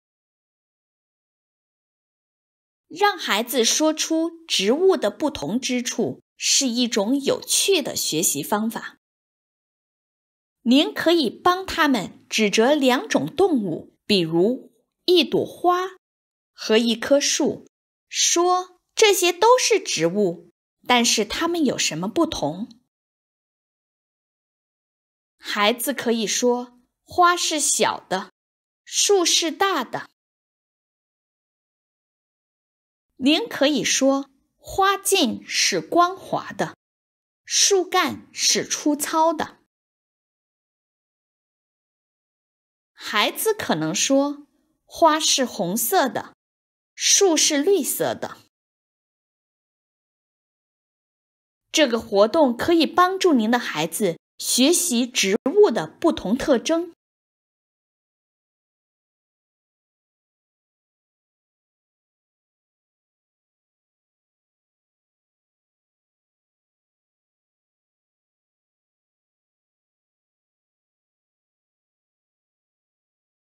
植物之间的不同 - Differences Between Plants (Chinese)
你可以用描述的字去比较两种植物，去帮助孩子学习植物和它们的特征。

Chinese translation by Ming Liu, and Xueni Peng. Narration by Xueni Peng.

You can help a child learn about plants and their characteristics when you compare two types of plants and use descriptive words.